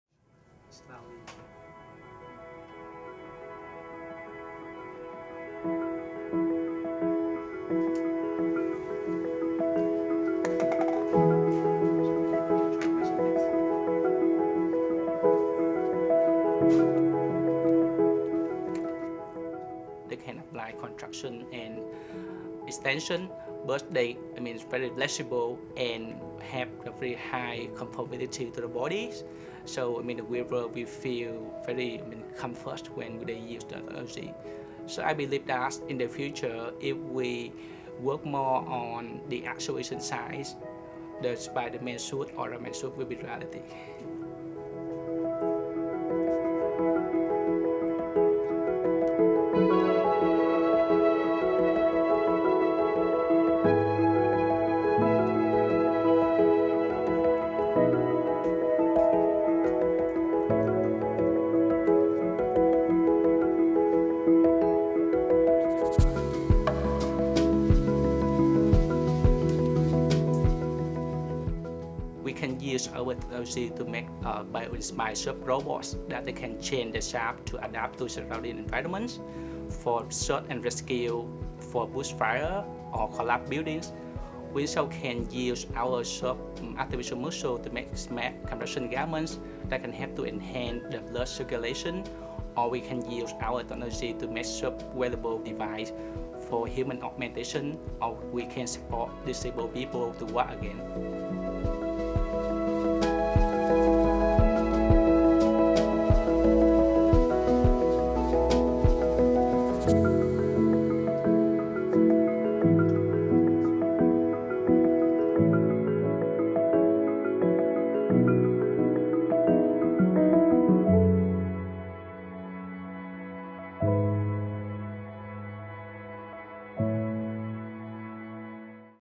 Smart textile acts like biological muscle, similar to Spider-Man suit
A team of biomedical engineers in Australia has developed a smart textile that incorporates artificial “muscles”, in hopes that it will revolutionise the future of soft robotics and wearable exoskeletons. The team demonstrated the shape-morphing flexibility of the material with models of a butterfly and flower made from the smart textile, performing complex motions in the lab. Team leader Thanh Nho Do has bigger plans and believes that his team will eventually develop a human smart suit similar to the Iron Man and Spider-Man suits from the Marvel Universe.